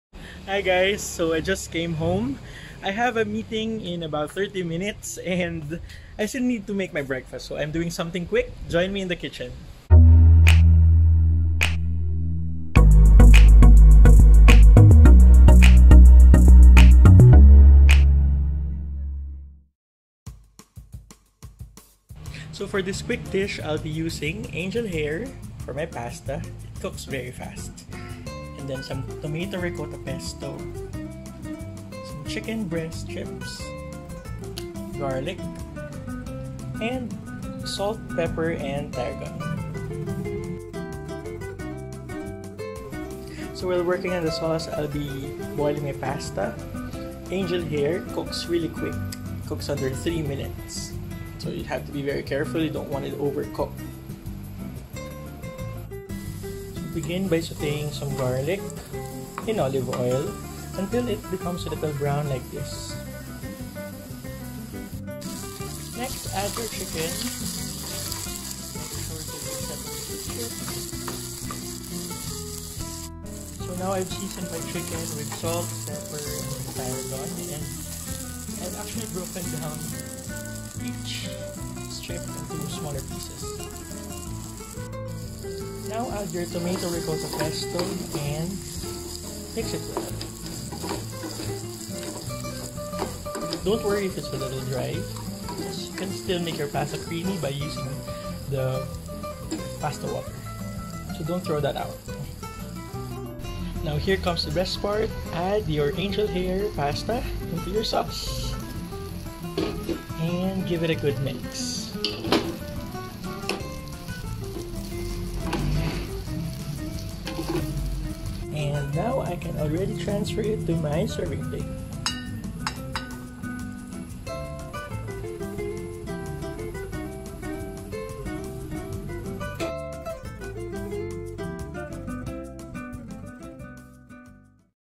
Cooking vlog 06 | Capellini with chicken and tomato & Ricotta pesto
In a hurry? This pasta dish takes less than 15 minutes to prepare!